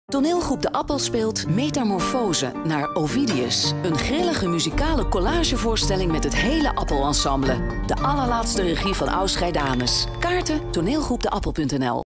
Metamorfosen - STER
Ster spot voor Metamorfosen.

Metamorfosen
tekst Aus Greidanus en Jules Terlingen
regie Aus Greidanus
spel Petra Ball, Nadine Bastiaansen, Marguerite de Brauw, Isabella Chapel, David Geysen, Geert de Jong, Judith Linssen, Hugo Maerten, Saskia Mees, Christina Schönbach, Bob Schwarze en Iwan Walhain
decor Guus van Geffen
kostuums Marrit van der Burgt
licht Guus van Geffen en Henry van Niel
muziek en geluid Carl Beukman en Saskia Mees
componist zangnummers en repetitor Saskia Mees
dramaturgie Alain Pringels
speelperiode woensdag 22 oktober t/m zondag 15 februari 2015